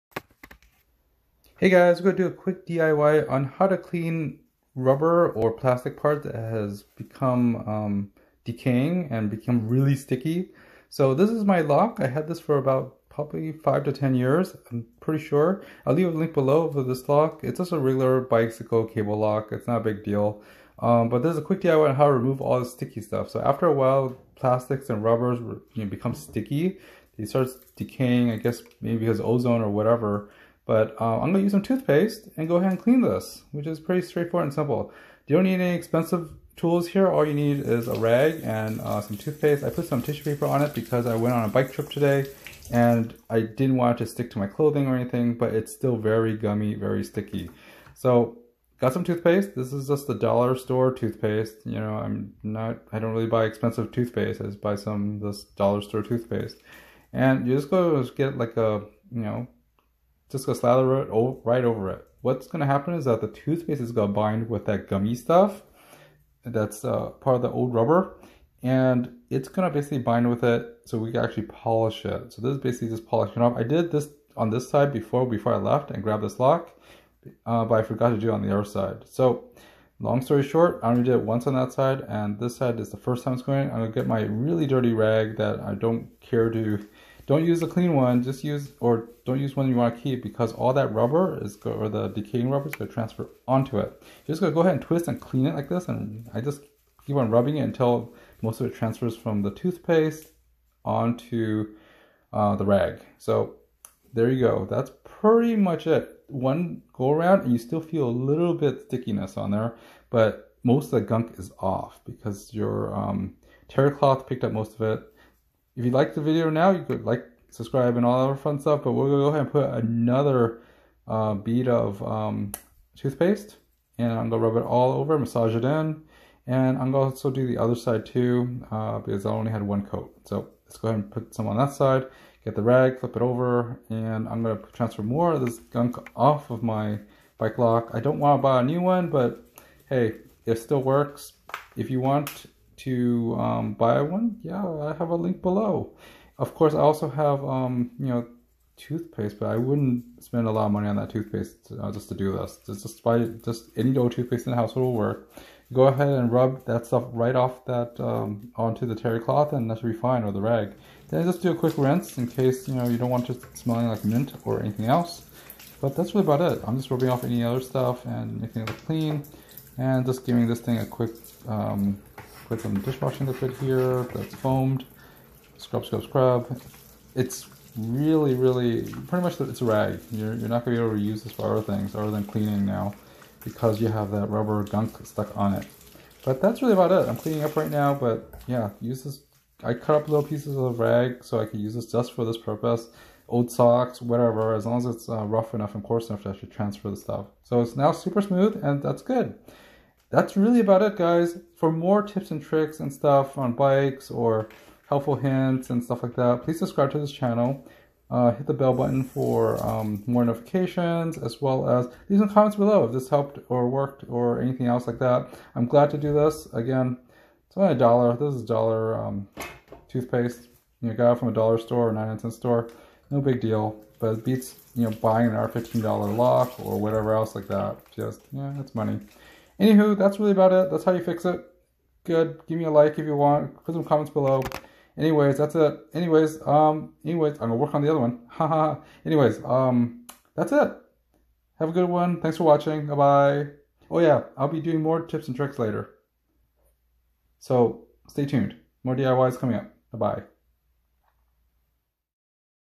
DIY: HOW TO REMOVE OLD STICKY RUBBER with TOOTHPASTE!!!
CLEANING DIY: REMOVING STICKY RUBBER from old rubber or plastic.

So you have old rubber or plastic that has become sticky and gummy. I had an old ON GUARD (OG) bike cable lock i got years ago ) with rubber parts starting to break down and got sticky.

I made a quick video on how to remove old sticky rubber from rubber parts and plastic.  All you need is TOOTHPASTE!!!  Any cheap too paste will do ) and a terry cloth or box of shop rags ) you plan to use get dirty or throw away later.   I used a whitening toothpaste from my bathroom and an old terry rag.

You want to put the toothpaste on the sticky rubber. Massage and coat the sticky parts. Then use a DRY terry cloth rag to rub and polish the toothpaste and sticky rubber off. This will remove 70%-80% of the stickiness. Put a second coat on, and use another side of the terry cloth and rub the toothpaste and sticky rubber off again.  After the second pass, the plastic should clean. Do a third pass if you like.

Then rinse off with water and test the stickiness. It should be clean and polished with the toothpaste. Most of the gummy rubber will transfer to the rag with the toothpaste.
Wash and reuse the rag, or clean more sticky items before disposing of the rag.

DONE! Simple, and no harsh chemicals!